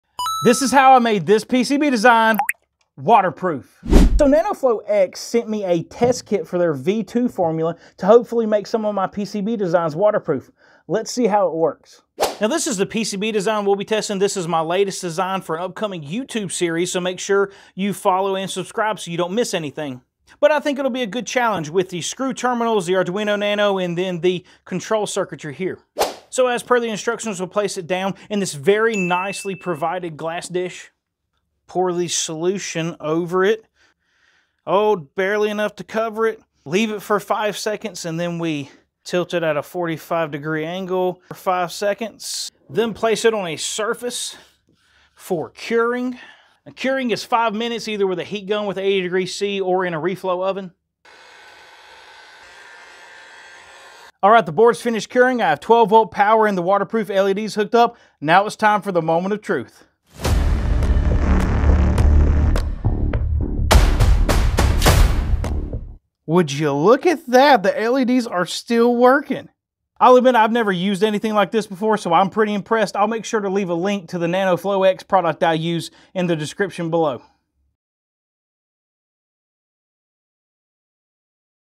WaterProofing My PCB Designs!!!
Testing waterproofing on one of my PCB designs.

NanoflowX sent their V2 coating, so I threw it at a board full of “don’t-submerge-this” parts: buttons, switches, a potentiometer, an Arduino Nano, and screw terminals with bare 12V + WS2811 wires.

Coated it, dropped it in a fish tank… no shorts, no glitches.

Pretty impressed.